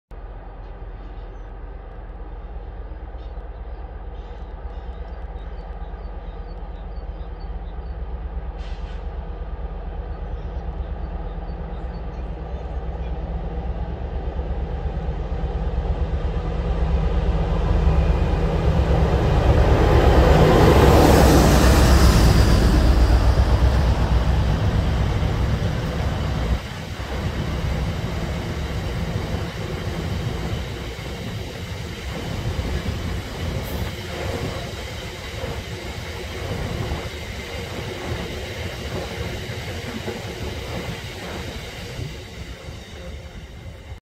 DC Rail Freight 60028 working Middleton Towers to Ravenshead Sidings 11/2/2023
Great Central Railway

Class 101 DMU - E50266 & 50203

Working the lunchtime service from Loughborough GCR Station to Leicester North Station

11/2/2023